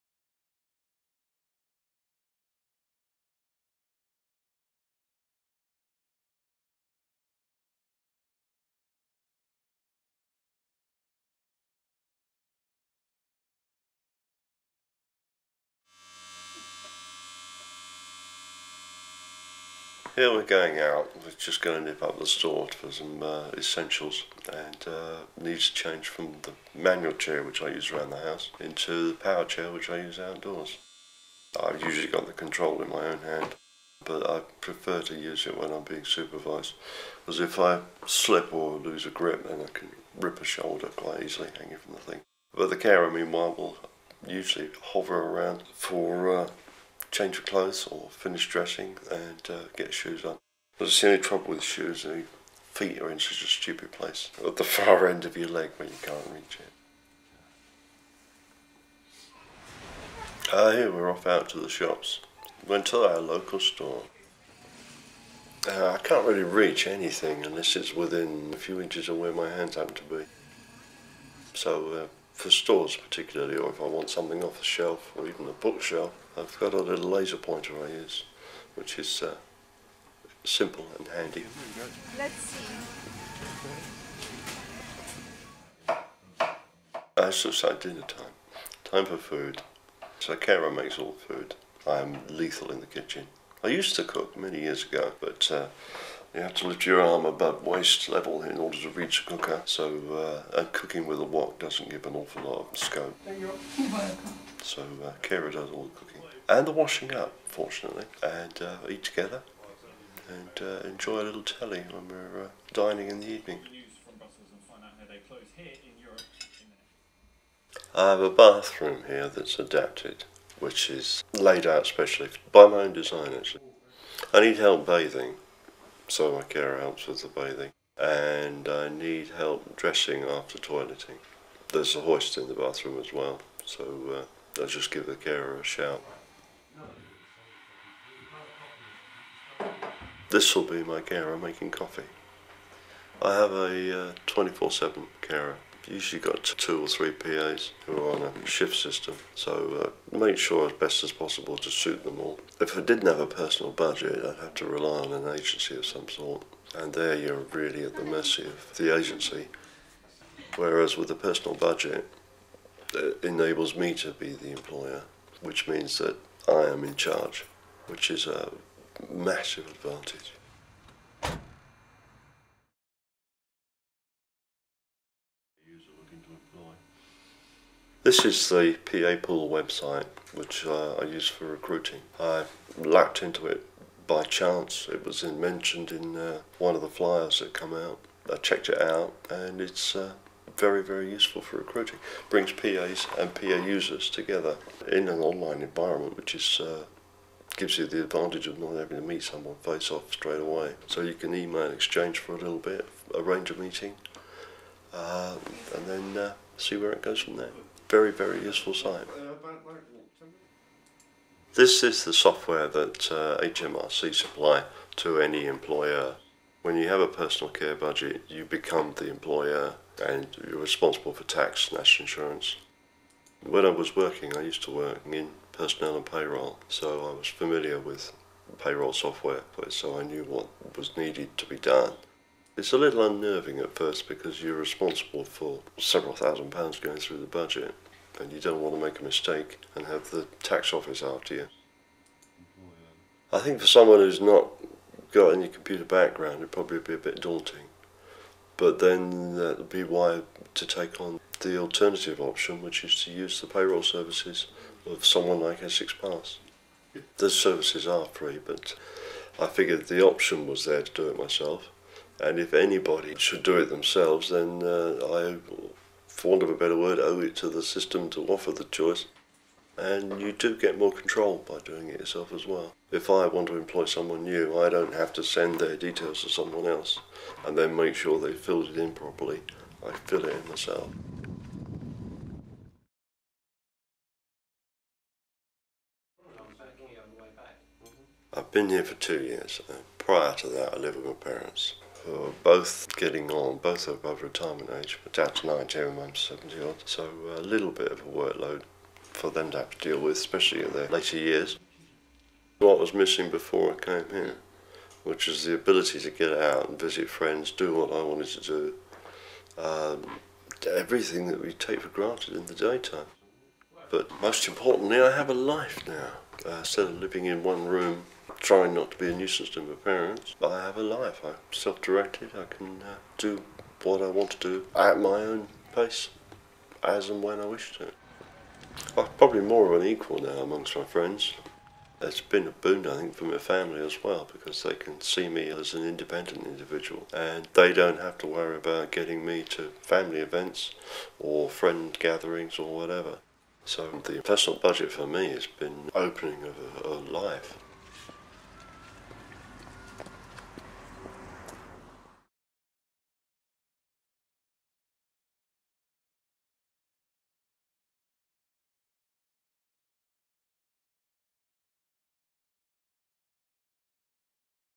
Essex longitudinal study on Personal Budgets -- Simon's story (subtitles)
In 2008, Essex County Council (ECC) commissioned ecdp and OPM to follow people over 3 years as they use cash payments for adult social care within Essex.

This study provides a unique opportunity to fully understand the experiences of people living with a personal budget over this time - a perspective that is often overlooked.

This video shares the lived experience of one of the people we have been following over the last 3 years.

You can read the full, final report, 5 associated briefing papers and see the other 2 videos in this series on ecdp's